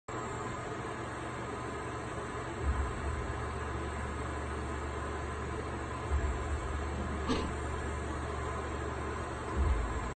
White in the nuts
Our little nut shot dec Bell with a trick shot to end all trick shots